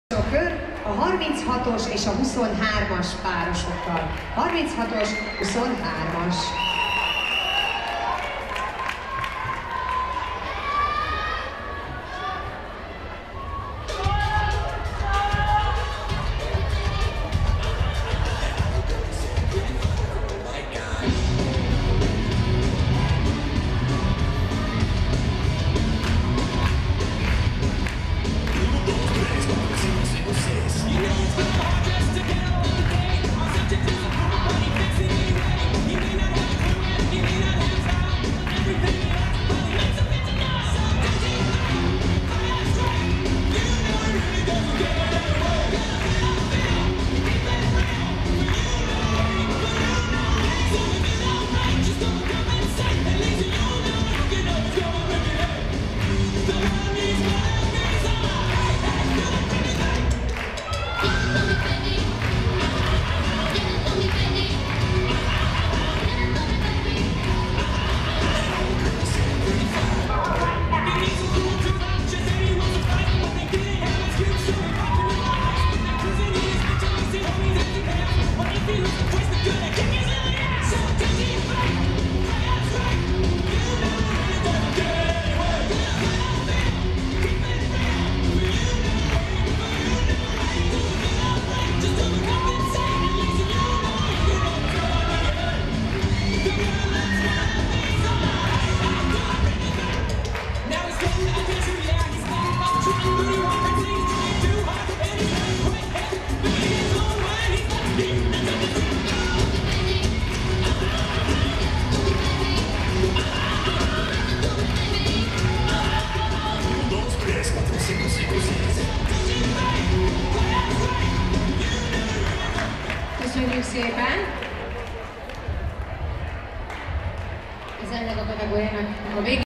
Deák Hunor-Szabó Alexandra MB 10.hely, 2017.12.10. Érd
akrobatikus rock'n'roll,
serdülő páros